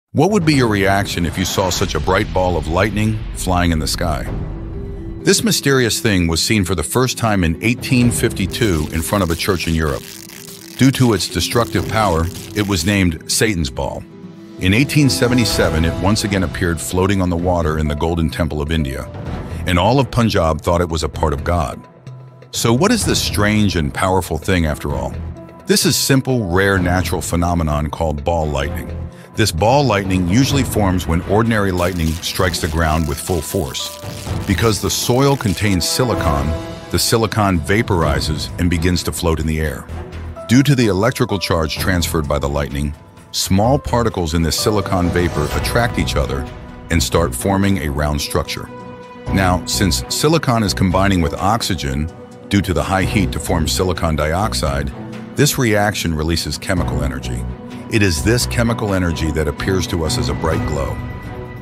"""Unveiling Ball Lightning: Nature's Fiery Phenomenon! ⚡🌍 ScienceFacts """
"What would you do if you saw a bright ball of lightning in the sky? First seen in 1852 in front of a church in Europe and later at the Golden Temple in India, this rare natural phenomenon is known as ball lightning. Formed when lightning strikes silicon-rich soil, it creates a glowing sphere of silicon dioxide. Learn more about this mysterious and powerful event!